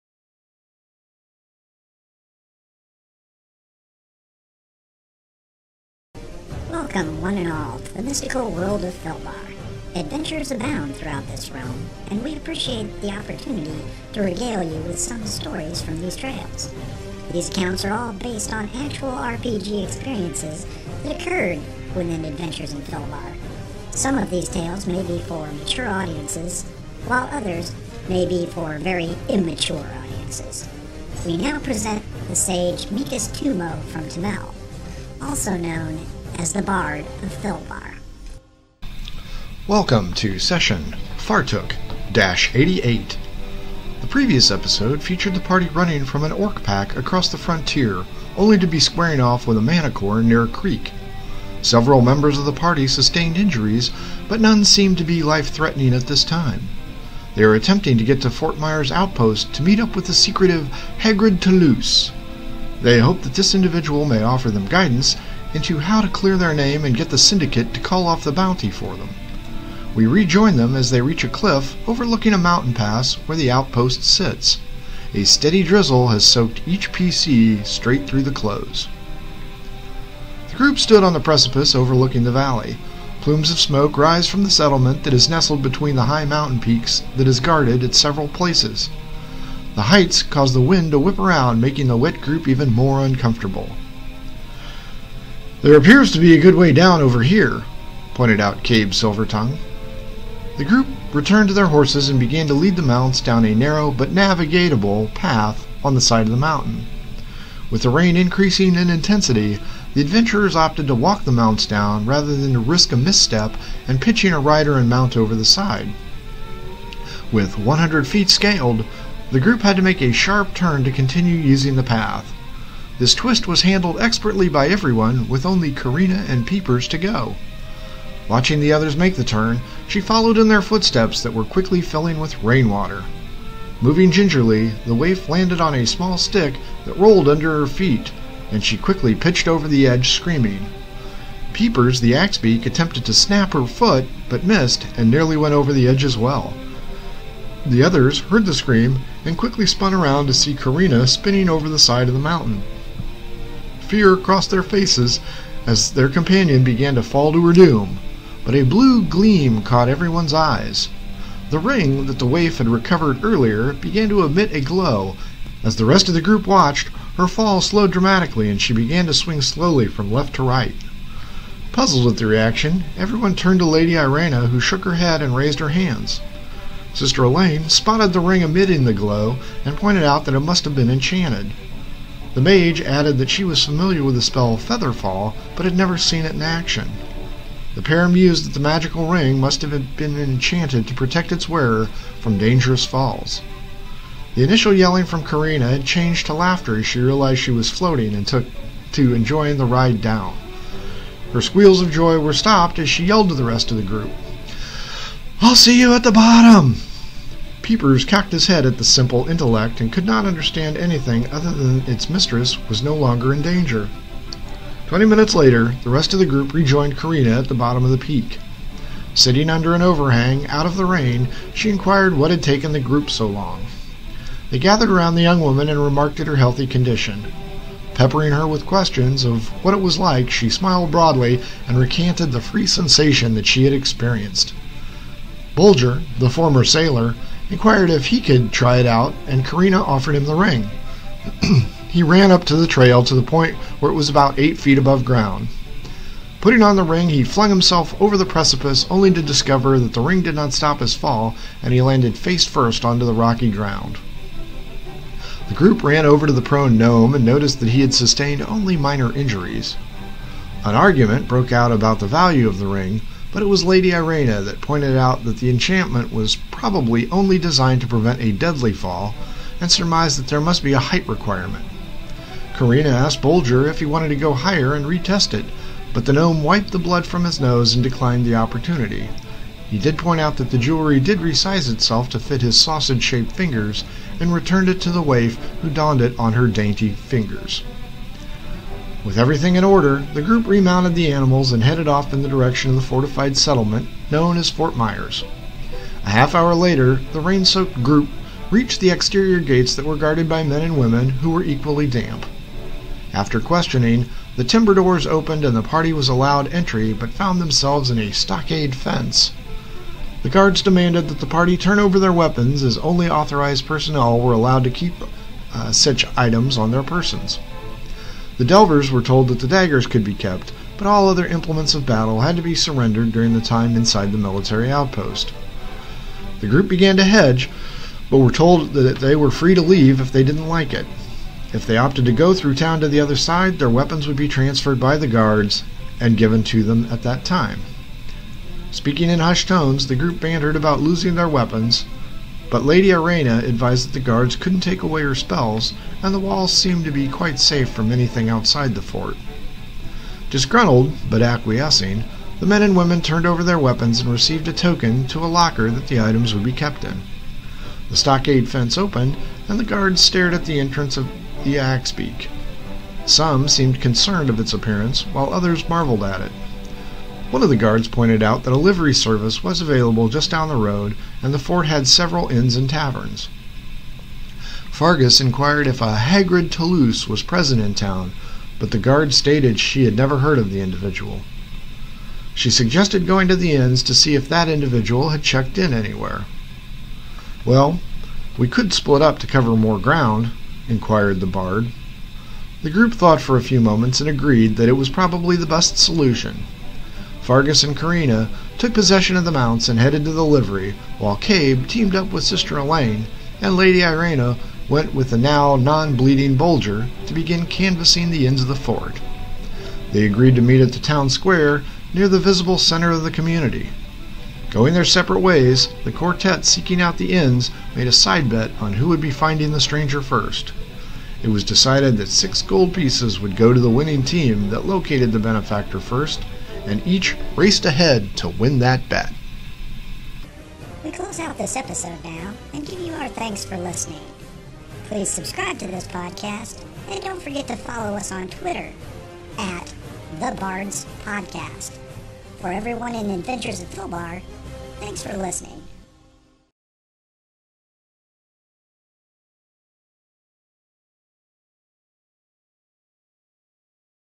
Session Fartook - 88 Fort Myers
The group discovers the magic of Karina's ring and arrives at Fort Myers!